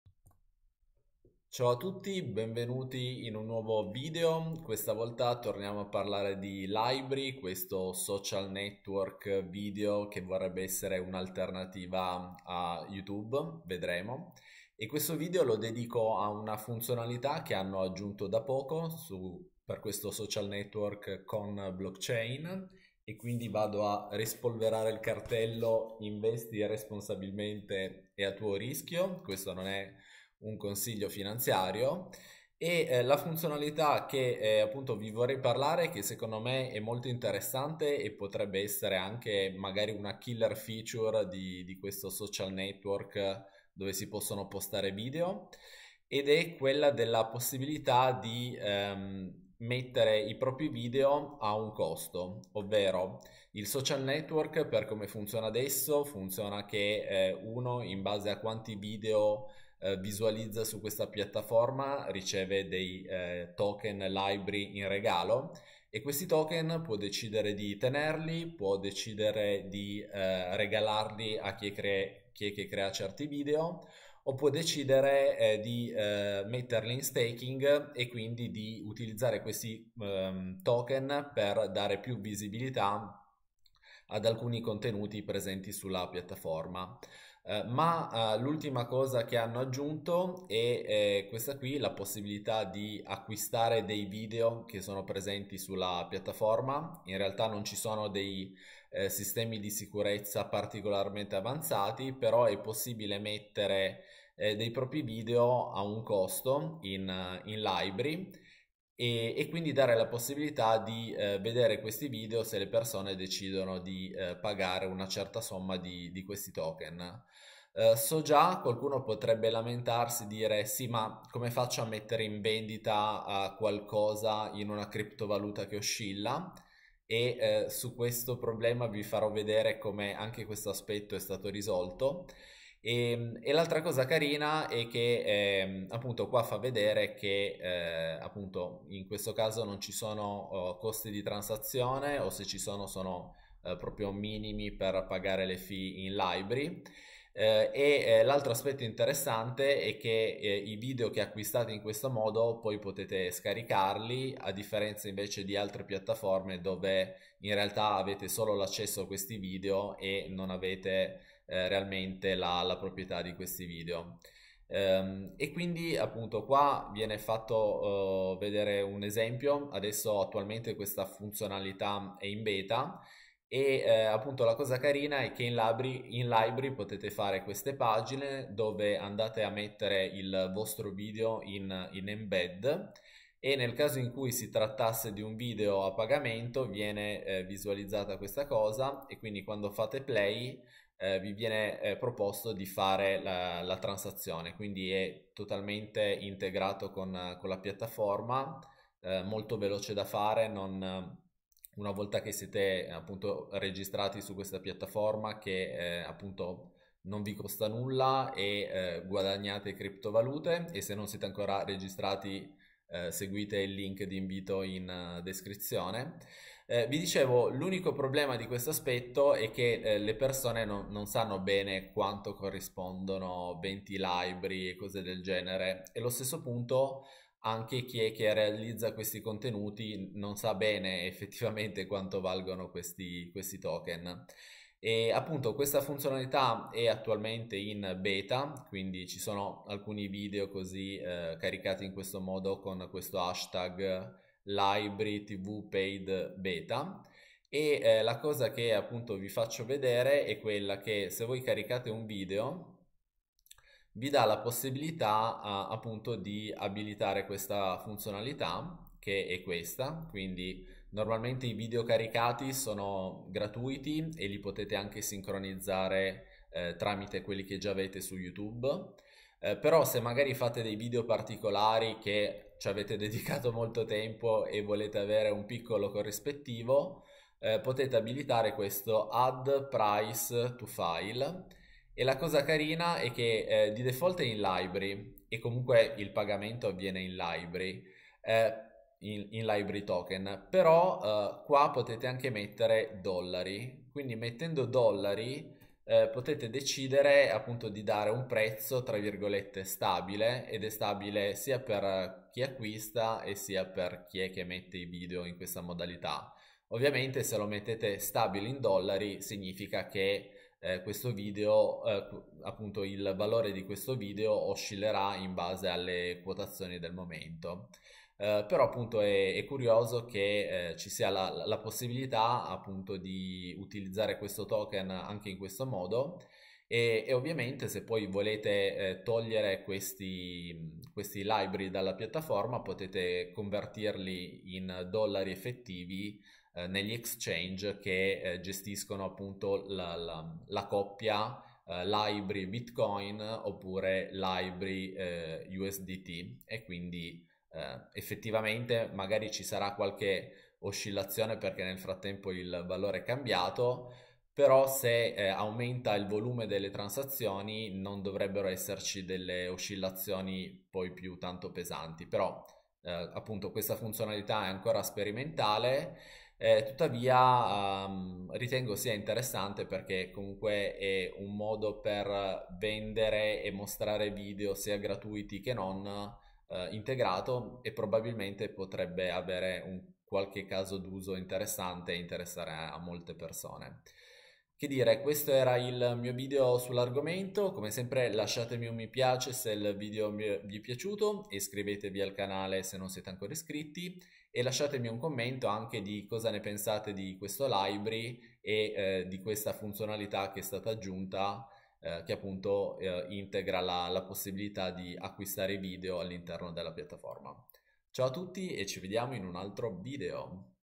💸 LBRY aggiunge una killer feature! Monetizzare video con 1 click!
LBRY è una piattaforma social blockchain dove le persone guadagnano crypto per guardare video o creare contenuti. Puoi abilitare la sincronizzazione automatica dei video dal tuo canale YouTube e da poco è possibile anche vendere i video con 1 click!